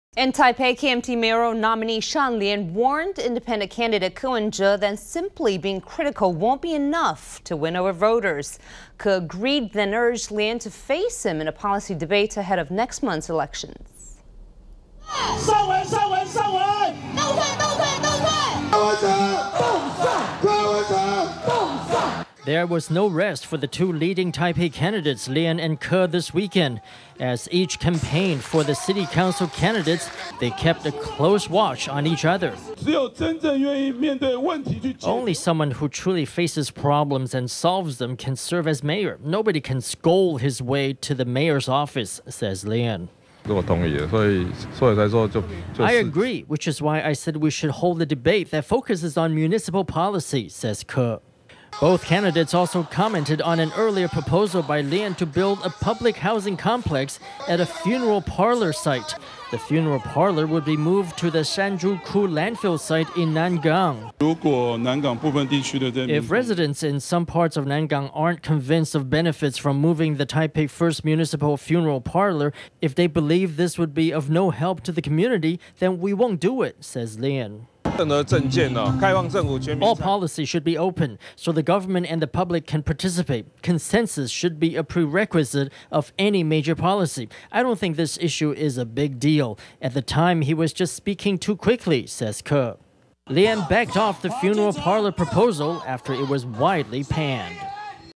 KMT’s Lien says Ko’s negative approach won’t win over voters
In Taipei, KMT mayoral nominee Sean Lien warned independent candidate Ko Wen-je that simply being critical won’t be enough to win over voters. Ko agreed then urged Lien to face him in a policy debate ahead of next month’s elections. There was no rest for leading Taipei candidates Lien and Ko this weekend. As each stumped for city council candidates, they kept a close watch on each other.Sean LienKMT Taipei Mayoral NomineeOnly someone who truly faces problems and solves them can serve as mayor. Nobody can scold his way to the mayor’s office, says Lien.Ko Wen-jeIndependent Taipei CandidateI agree, which is why I said we should hold a debate focused on municipal policy, says Ko.Both candidates also commented on an earlier proposal by Lien to build a public housing complex at a funeral parlor site. The funeral parlor would be moved to the Shanzhuku Landfill Site in Nangang.Sean LienKMT Taipei Mayoral NomineeIf residents in some parts of Nangang aren’t convinced of benefits from moving the Taipei First Municipal Funeral Parlor, if they believe this would be of no help to the community, then we won’t do it, says Lien.Ko Wen-jeIndependent Taipei CandidateAll policy should be open, so the government and the public can participate. Consensus should be a prerequisite of any major policy. I don’t think this issue is a big deal. At the time he was just speaking too quickly, says Ko.Lien backed off the funeral parlor proposal after it was widely panned.